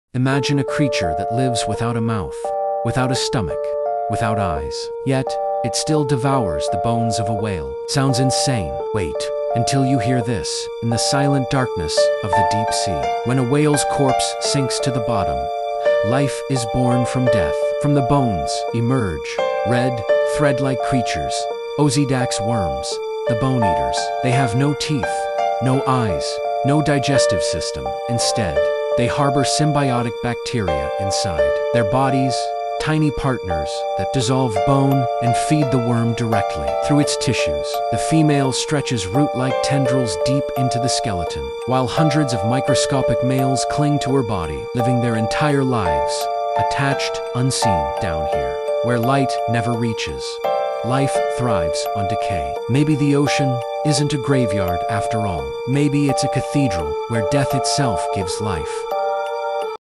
This Sea Creature Lives Without a Mouth, Without a Stomach, Without Eyes!
The Osedax worm feeds on whale bones at the bottom of the ocean — without a mouth! But how does it survive without any vital organs?